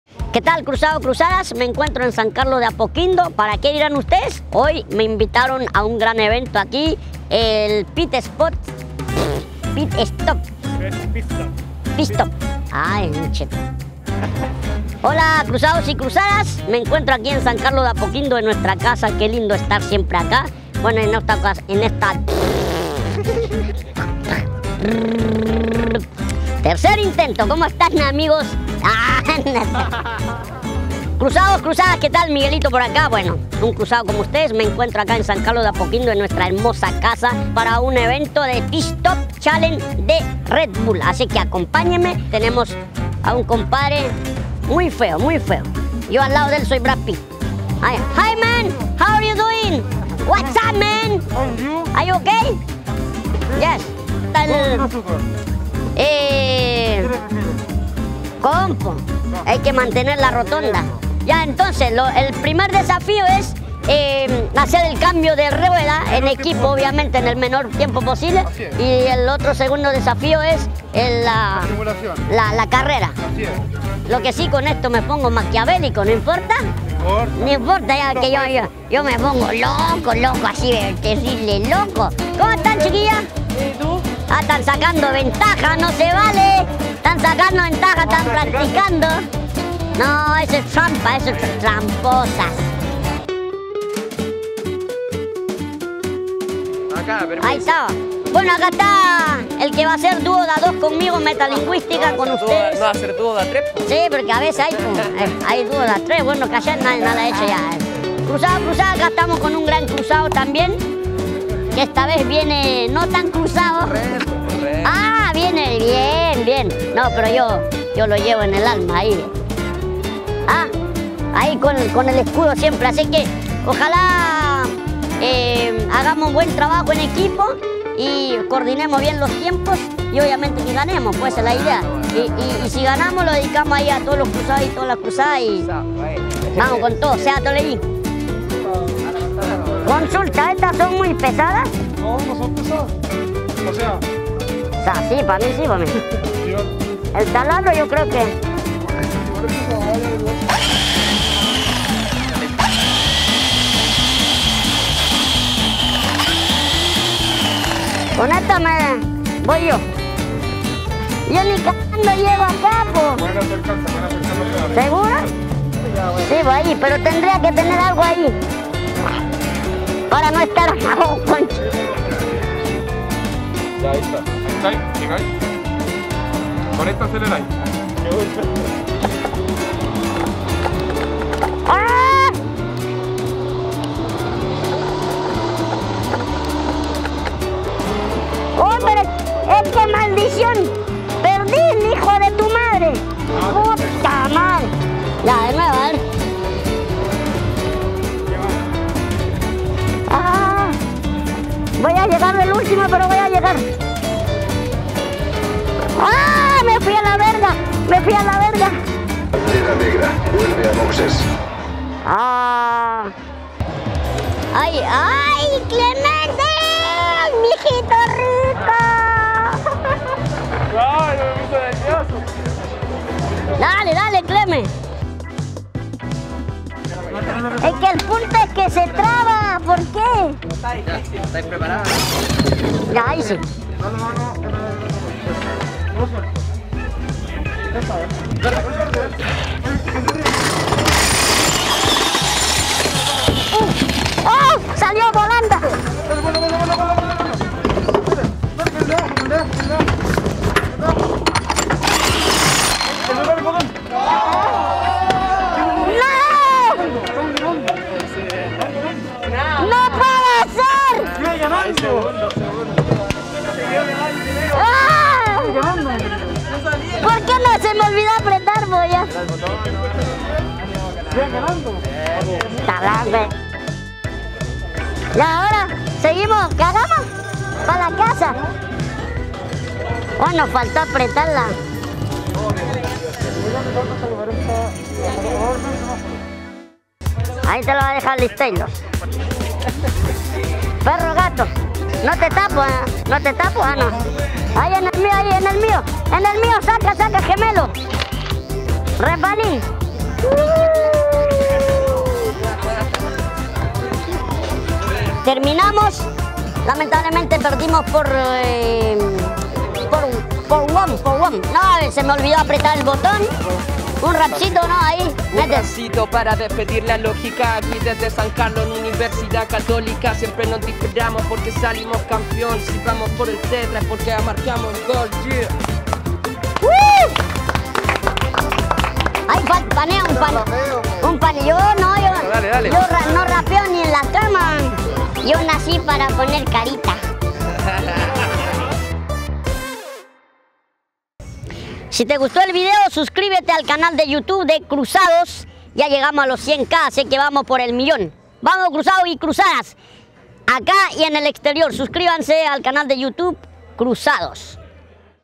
MIGUELITO SE LUCIÓ COMO PILOTO DE F1 🏁⚡ | ⚪🔵 Cruzados
Miguelito estuvo en San Carlos de Apoquindo para disfrutar del Red Bull Pit Stop Challenge, y vivió la experiencia de ser un pilóto de Fórmula Uno.

Allí, el actor y comediante compartió junto a Antonia Canales, Helaine de Grange, Vicente Bernedo, Clemente Montes, Cristobal Finch y el músico Metalingüistica